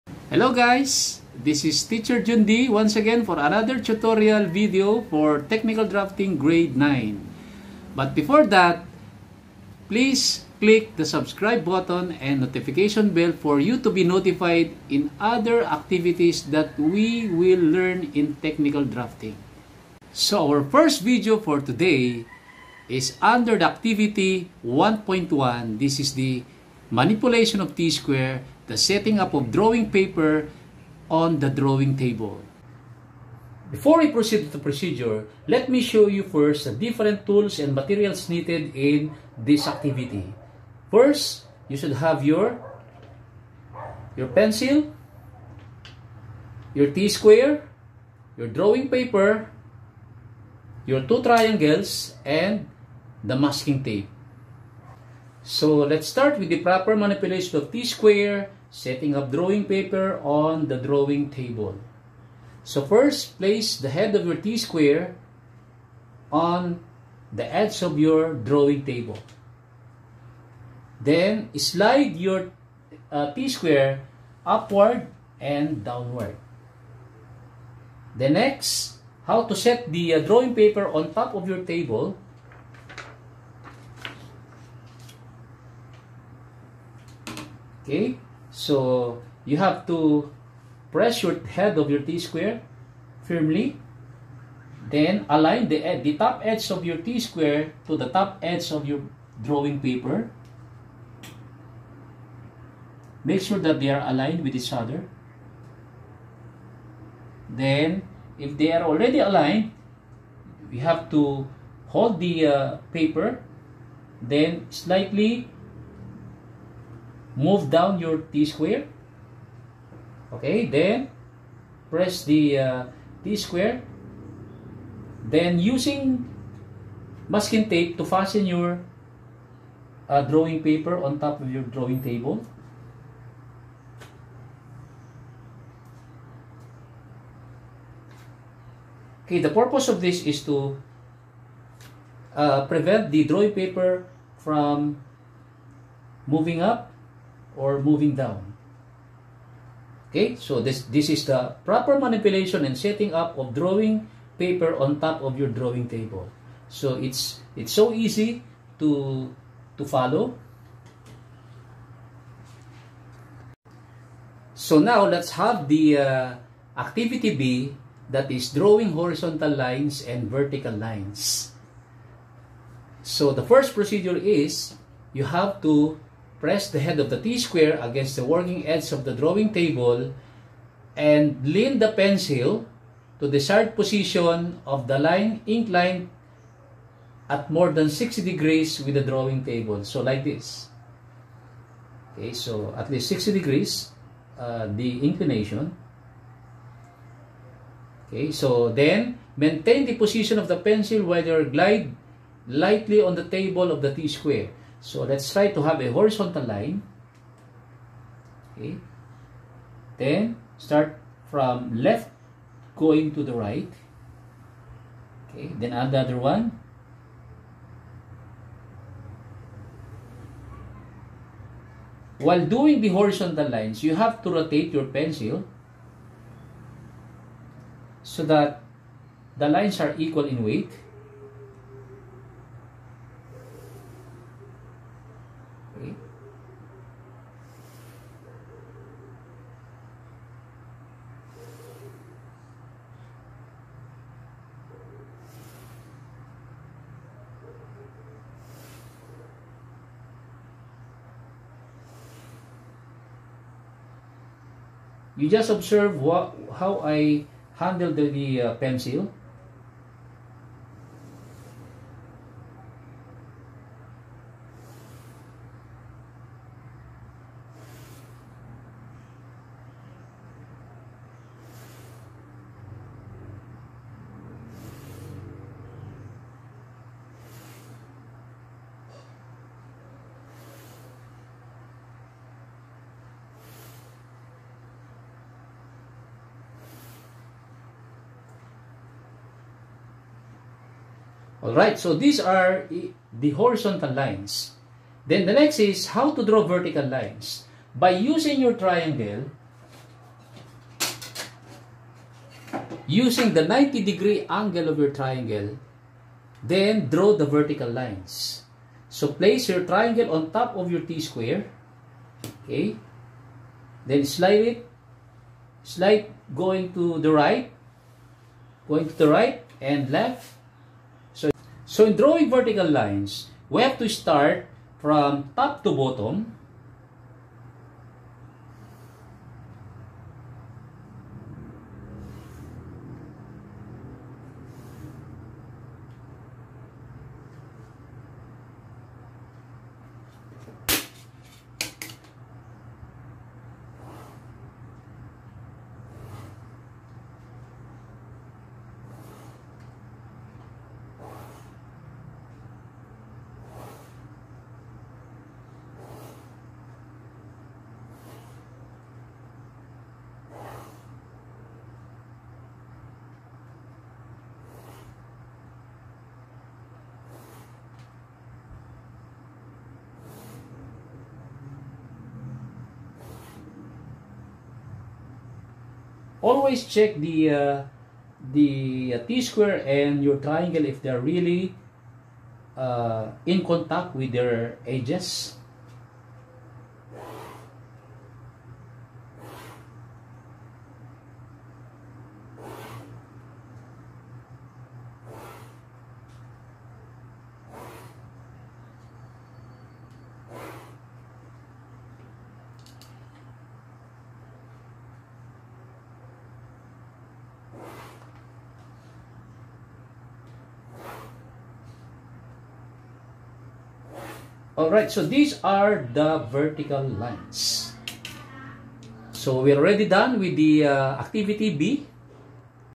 Technical Drafting Activity 1.1 A and B
A. Manipulation of T-square and setting up drawing paper on the drawing table.
B. Drawing of horizontal and vertical lines.